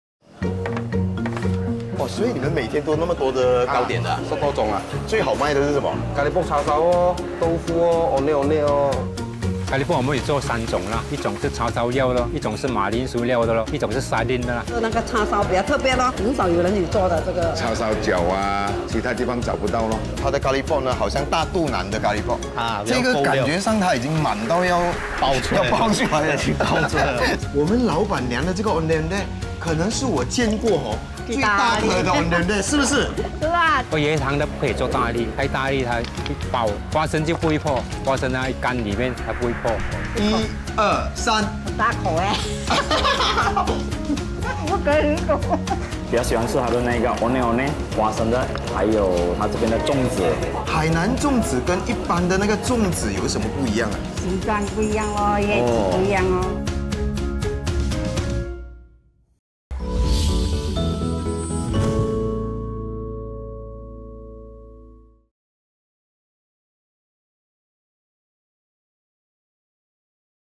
手作特大号澎湃好料 Genuine Generosity
店主天亮前必定到菜市亲选真材实料，这样才对得起老板娘的一双巧手，她能变出巧妙罕见“大肚腩”叉烧咖喱角、鲔鱼角、娘惹糕点、海南粽子、酿豆腐等等。她下手很重众所周知，她专门擅长做馅多料厚的特大号。对客人澎湃付出，成了嘉园茶餐室的经营之道。

At Jia Yuan, innovation is the order of the day. With some ingenuity, the lady boss has produced traditional snacks with a new twist, like char siew or tuna filled curry puffs. Add generosity with ingenuity, patrons flock to the shop for its jumbo-sized ondeonde, stuffed beancurd, Hainanese dumplings and more.

嘉园茶餐室Kedai Kopi Jia Yuan
10, Jalan 13, Kluang Johor
創始 Since 1989

重播Repeat： Mon 1130am，Tue 6pm，Sat 4pm